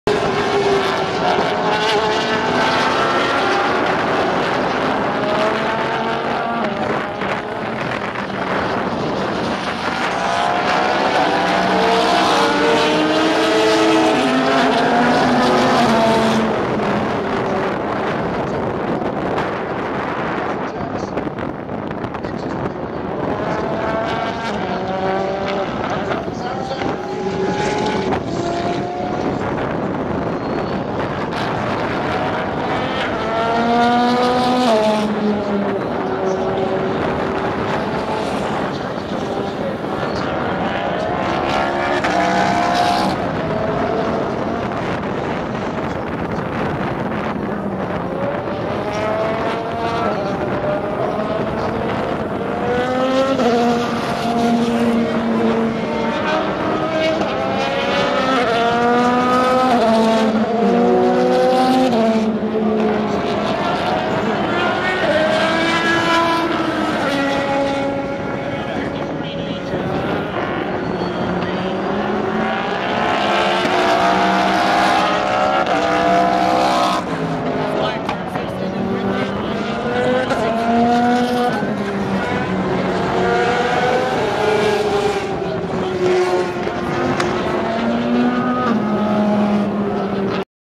ALMS Racing at Circuit of the Americas
Taken from the grandstands at turn 15.